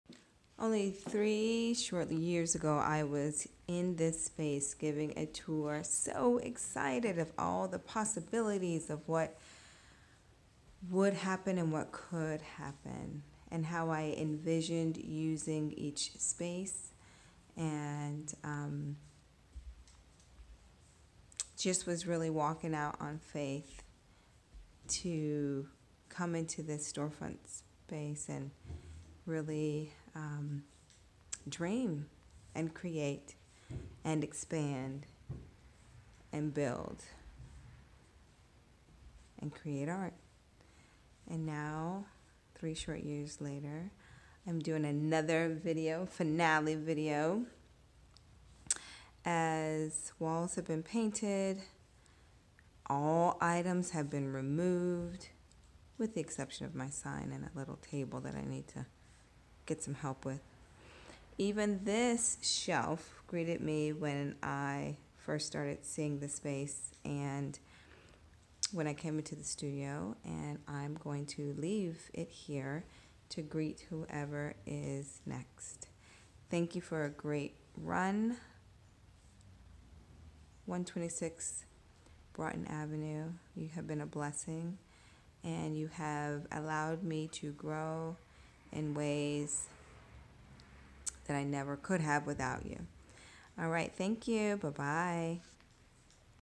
Lovlee Art Studios... Where creativity is expressed beyond the brush!
This is the exit video from my first Location! Such a blessing it was to creatively serve the community in this location!